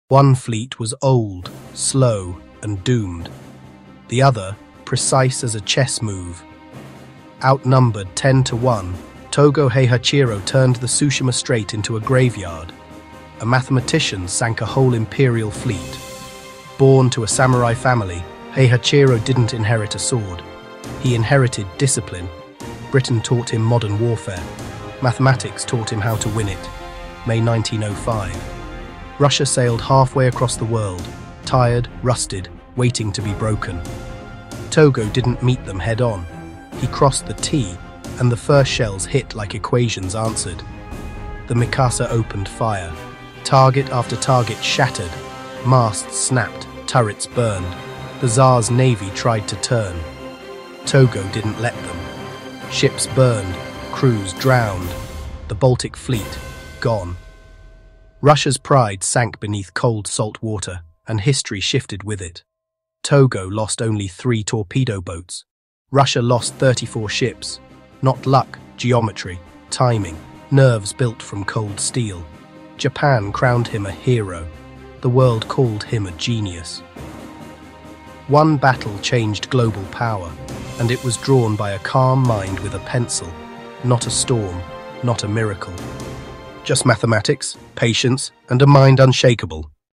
Outnumbered. Outgunned. Unshaken. Admiral Togo's Glory at Sushima
Outnumbered ten to one at Tsushima — and still he crushed the Russian fleet. Not through fury. Through mathematics.